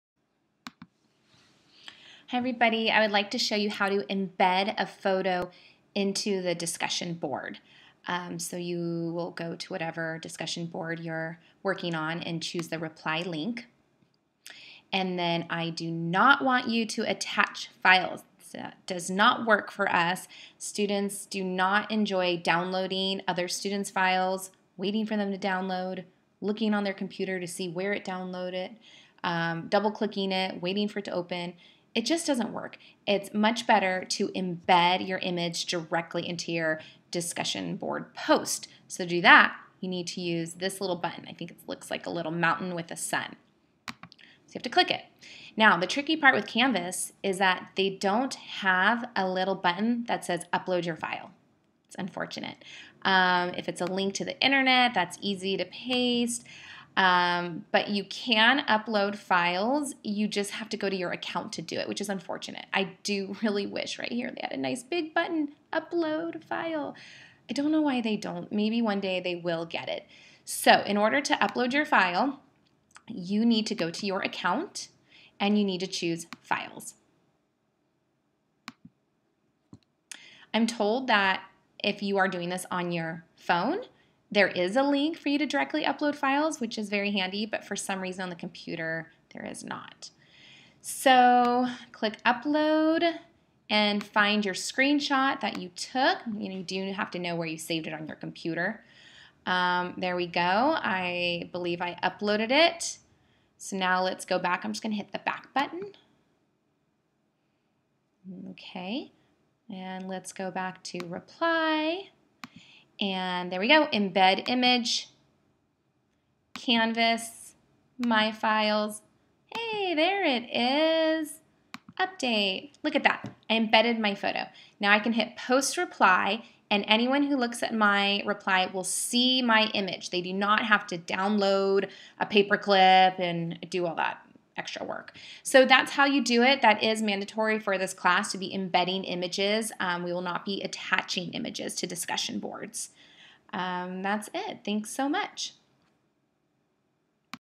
embed images into canvas
How to embed your screenshots directly into canvas discussion boards